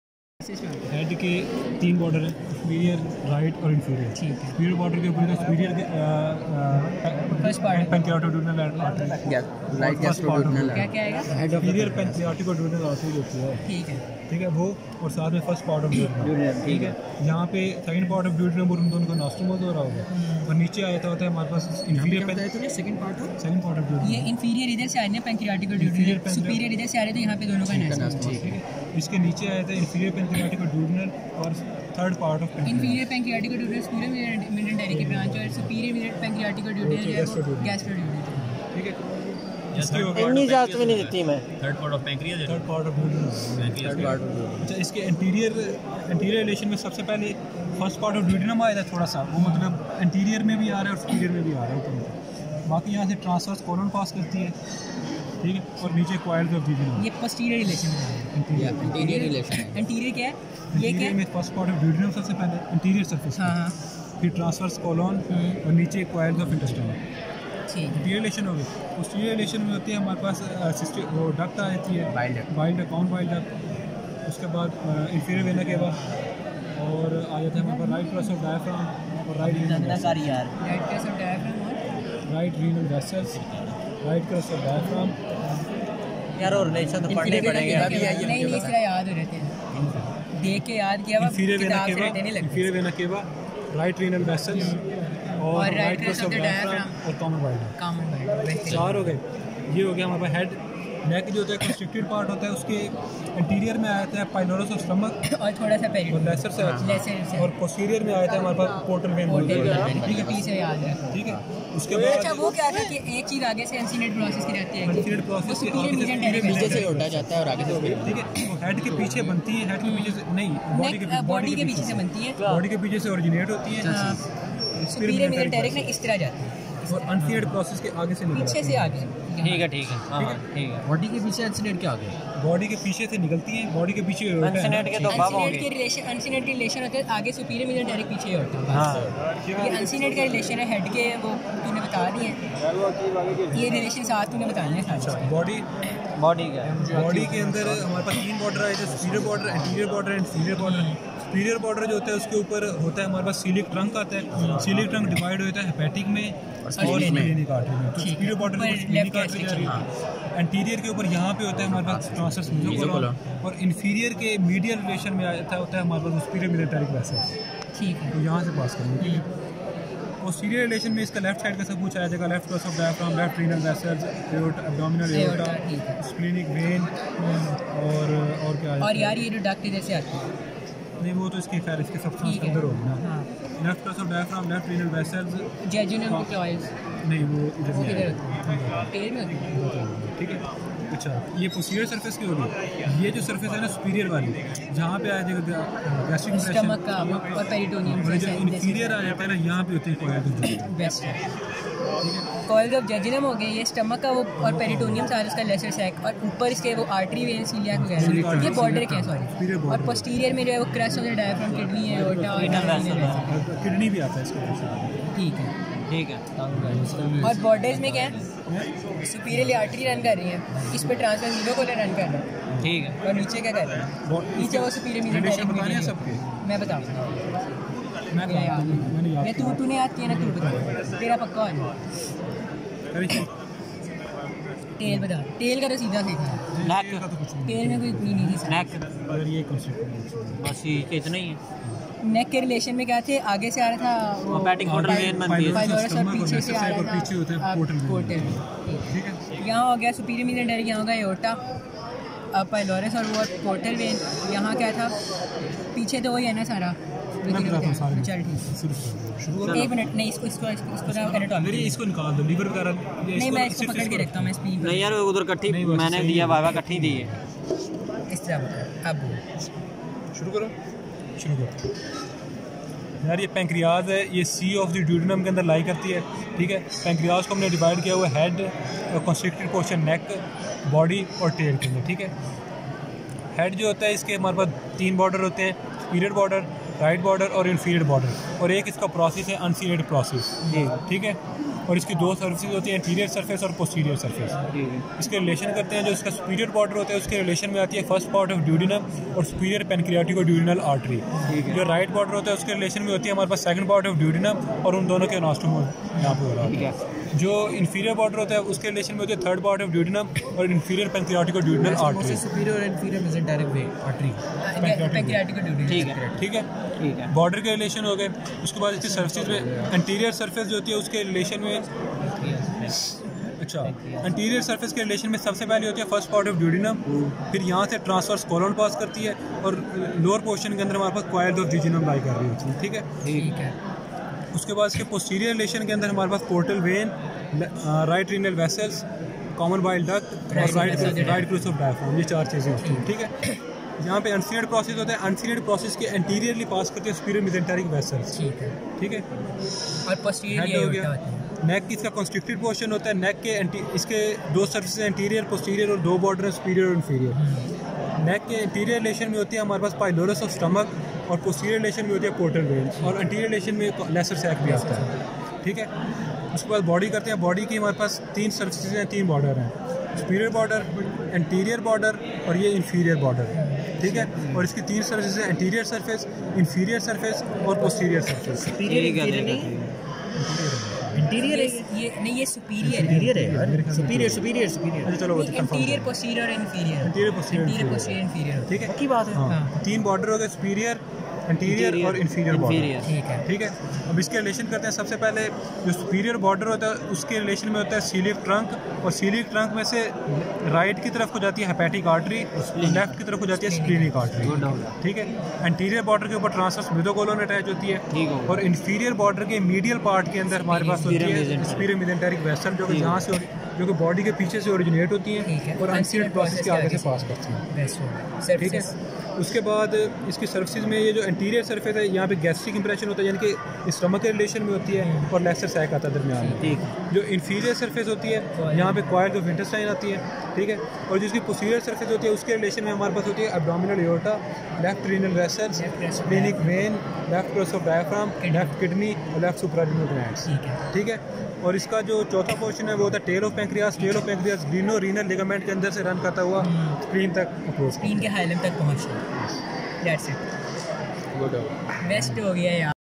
Abdomen and Pelvis Spotting (1)
Title:
Abdomen and Pelvis Spotting: Techniques and Tips for Effective Assessment

Description:
Welcome to our comprehensive guide on Abdomen and Pelvis Spotting! In this video, we will cover the techniques and tips for effective assessment in order to help you improve your spotting skills.

As a healthcare professional, it's crucial to accurately identify and diagnose potential issues in the abdomen and pelvis. Our expert instructors will walk you through a step-by-step approach to ensure you don't miss any critical findings.

From assessing organ size and shape to identifying abnormal masses, this video covers it all. We'll also discuss common pitfalls to avoid and provide tips for improving your overall spotting accuracy.

So whether you're a medical student, resident, or practicing physician, this video is a must-watch for anyone looking to improve their abdomen and pelvis spotting skills.